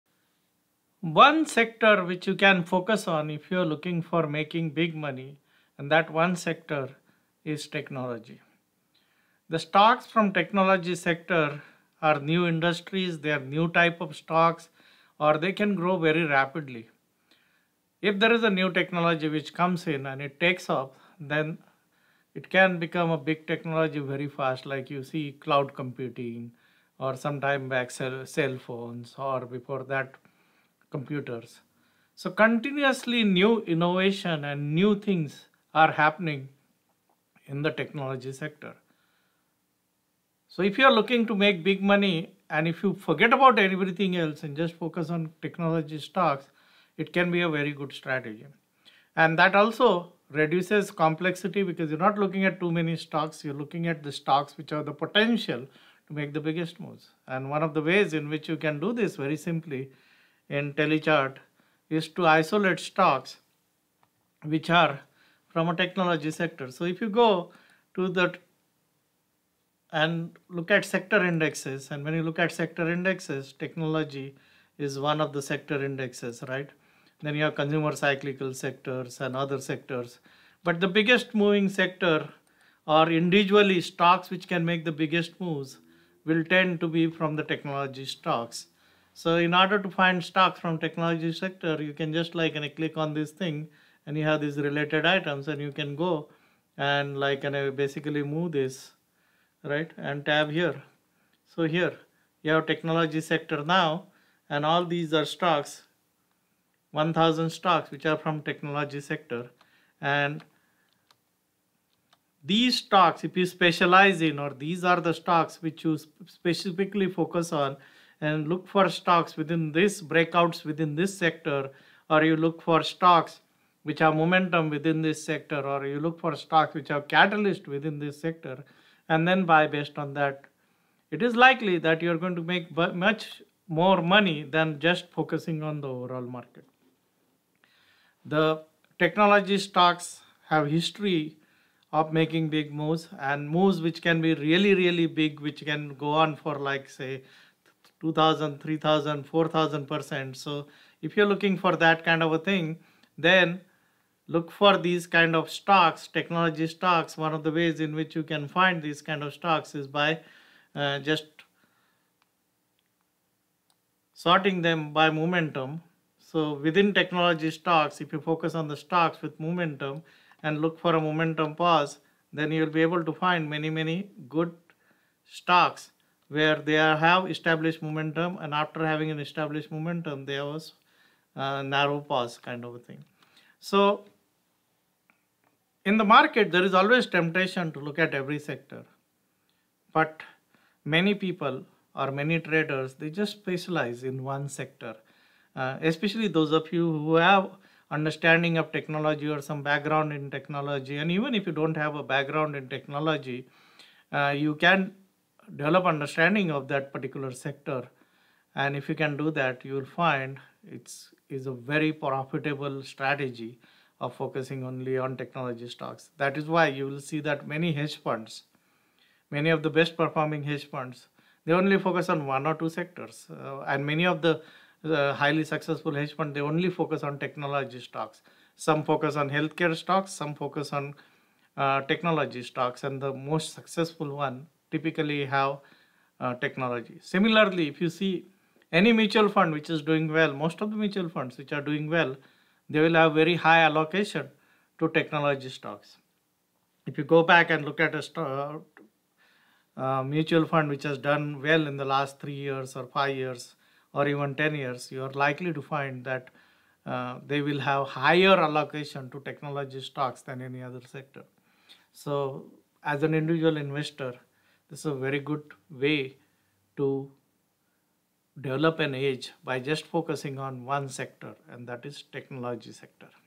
Focus only on one sector: Technology
If you want to make big money focus on technology stocks.